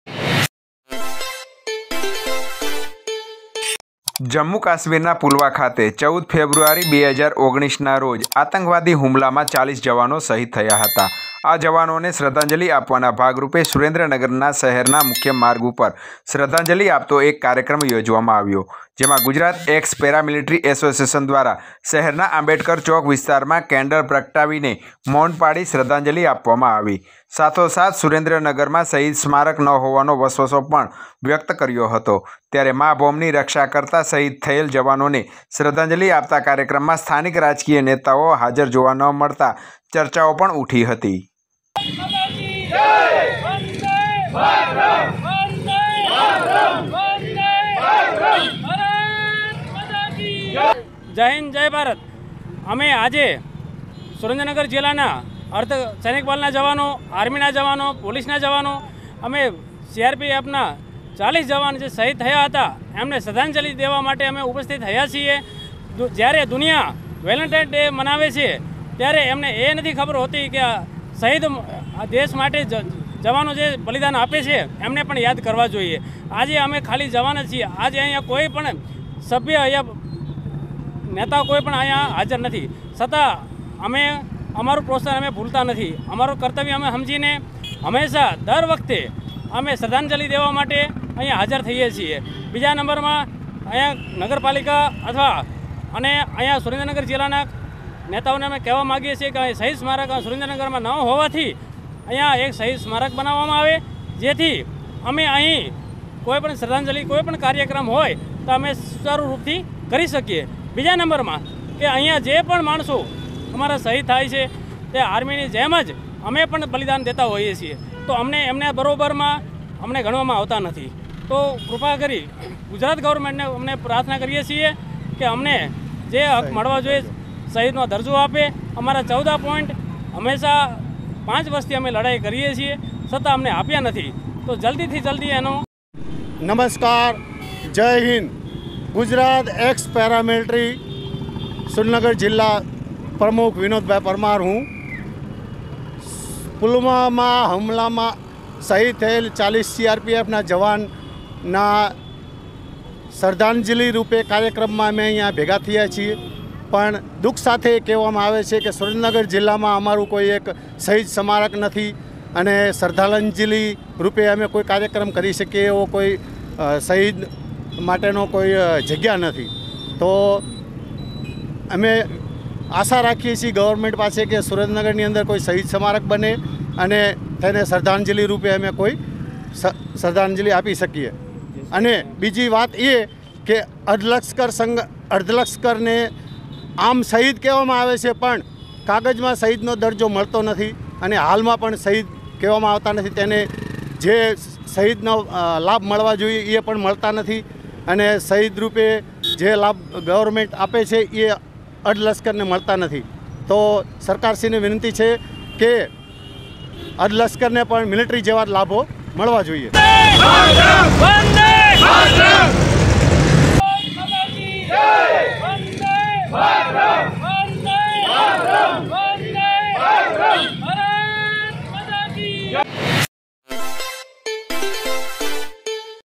Surendranagar શહીદોના સ્મારકની માગ...શ્રદ્ધાંજલિ આપી..
સુરેન્દ્રનગર
14 ફેબ્રુઆરી રોજ જમ્મુ કાશ્મીરના પુલવામાં આતંકવાદી હુમલામાં 40 જવાનો શહીદ થયા હતા.. આ જવાનોને શ્રદ્ધાંજલિ આપવાના ભાગરૂપે સુરેન્દ્રનગરમાં પણ શહેરના મુખ્ય માર્ગ ઉપર શ્રદ્ધાંજલિ આપતો કાર્યક્રમ યોજવામાં આવ્યો.. ગુજરાત એક્સ પેરામીલેટરી એસોસિએશન દ્વારા શહેરના આંબેડકર ચોક વિસ્તારમાં કેન્ડલ પ્રગટાવી મૌન પાળી શ્રદ્ધાંજલિ આપી હતી.. સાથોસાથ સુરેન્દ્રનગરમાં શહીદ સ્મારક ન હોવાનો વસવસો પણ વ્યક્ત કર્યો હતો.. ત્યારે મા ભોમ ની રક્ષા કરતા શહીદ થયેલ જવાનોને શ્રદ્ધાંજલિ આપતા કાર્યક્રમમાં સ્થાનિક રાજકીય નેતાઓ હાજર જોવા મળ્યા ન હતા..